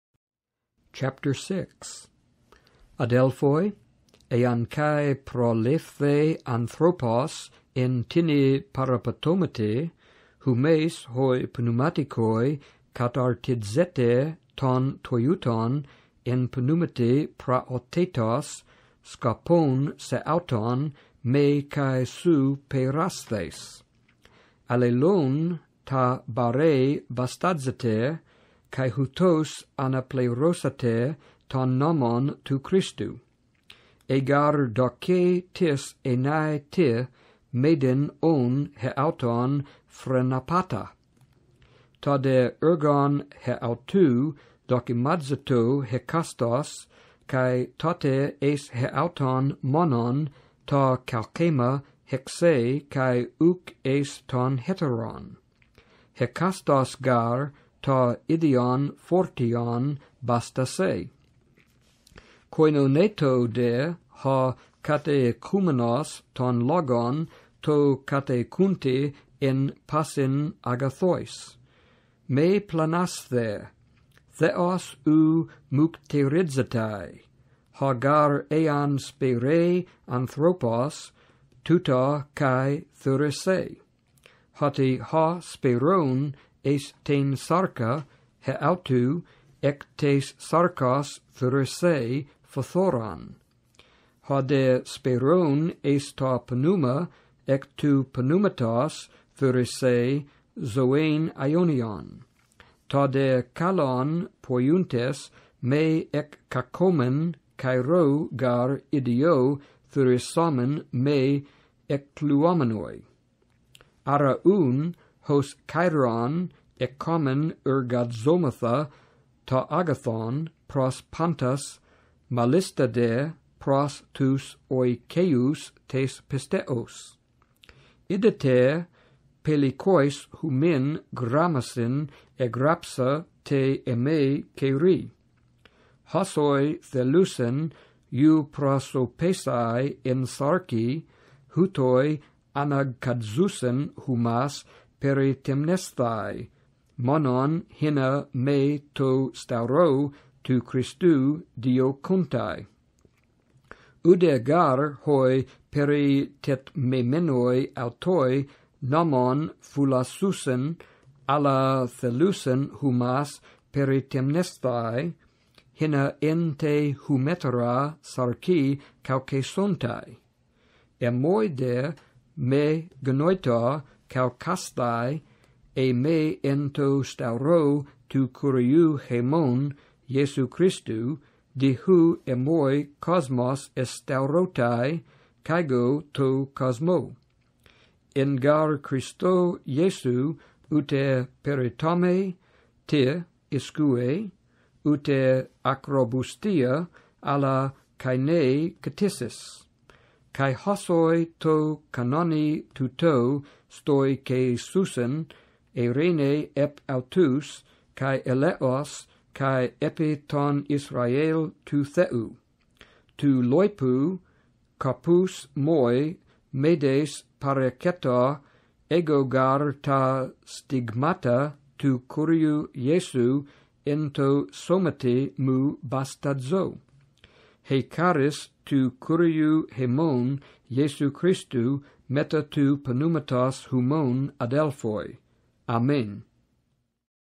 Galatians 6 in Greek (Maurice Robinson)
Maurice Robinson reeds Galatians 6 in Greek (Robinson-Pierpont 2005 edition).